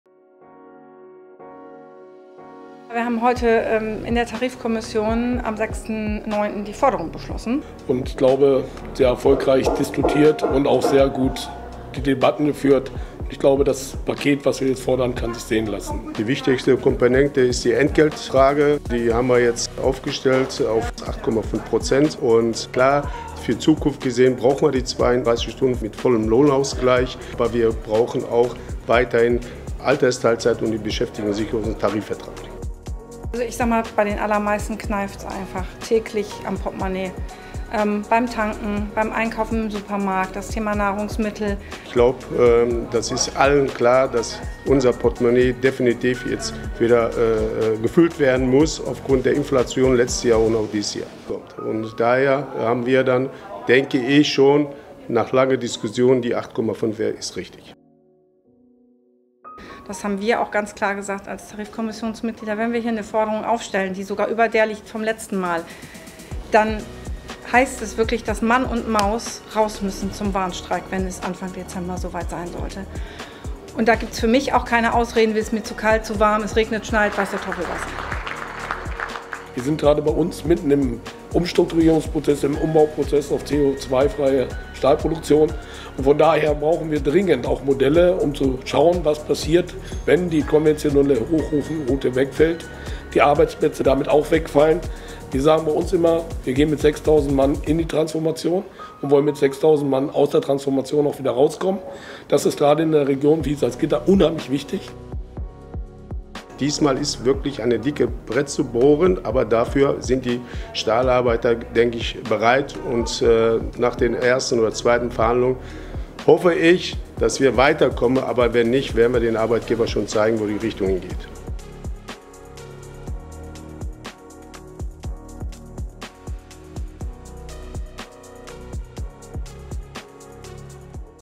Forderungsempfehlung Tarifrunde Stahl 2023
Nach intensiver Diskussion und einigen hitzigen Debatten steht die Forderungsempfehlung für die Tarifrunde der Eisen- und Stahlindustrie 2023. Was wir fordern? Erklären wir im Video.